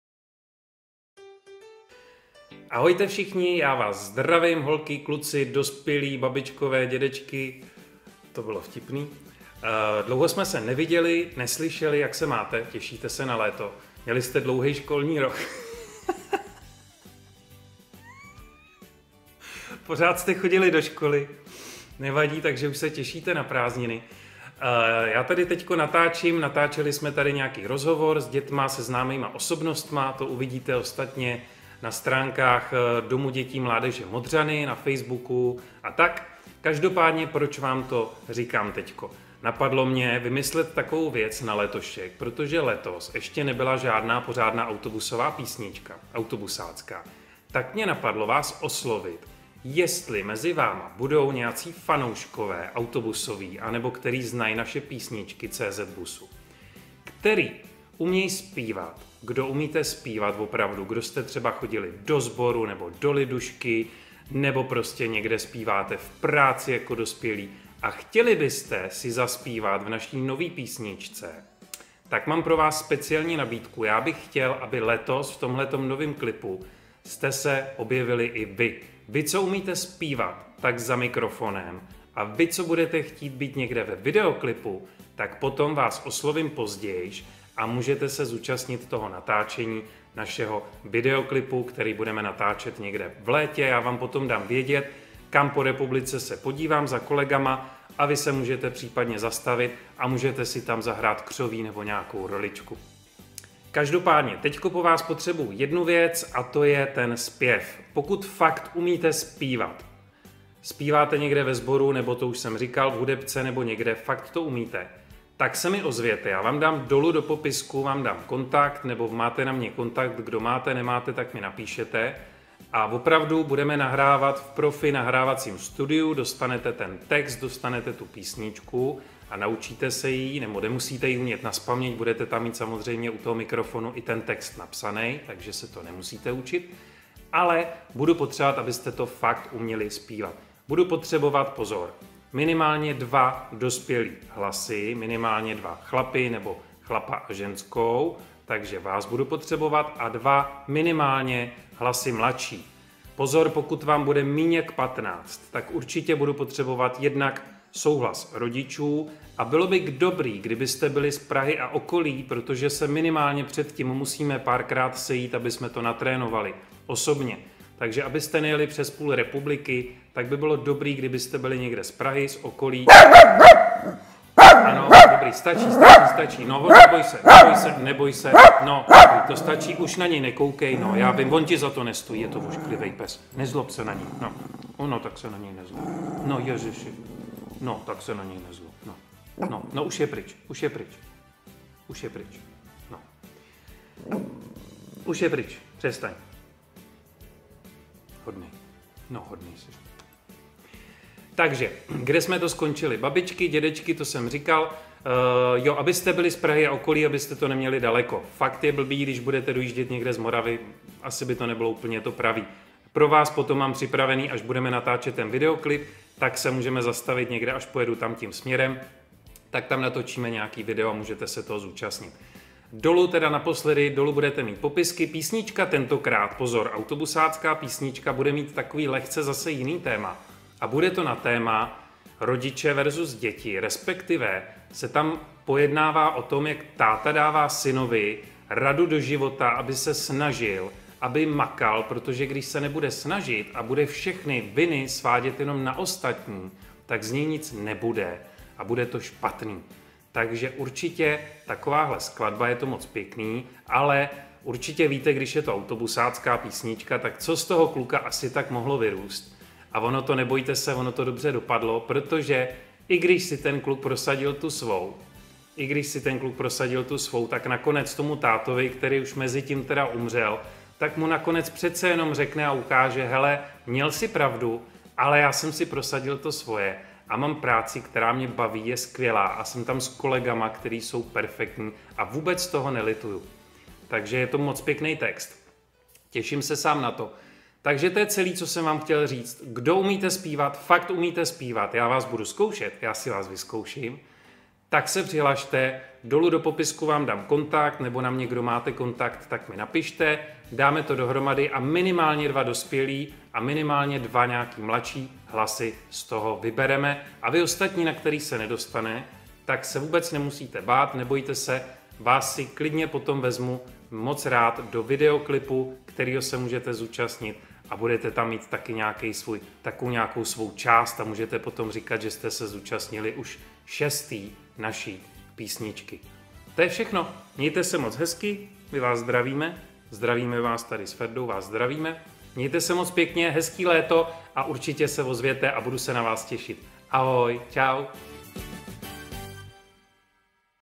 Zapojení fanoušků - KDO UMÍTE ZPÍVAT?
Umíš zpívat, máš rád(a) autobusy a chceš se mnou nazpívat další autobusáckou písničku?

Postupně někoho vybereme a nazpíváme společně další naši autobusáckou písničku.

POZOR! Je lepší být z Prahy a okolí kvůli zkoušení!
Je možnost po vybrání buď jen zpívat v nahrávacím studiu a nebo se objevit i ve videoklipu.

Ti, co na zpěv vybráni nebudete - zůstaňte v kontaktu na Instagramu - budou tam poté informace, kde se bude natáčet videoklip a můžete se přijít podívat či ve videoklipu i učinkovat :-)

Věřím, že i další písnička bude pecka!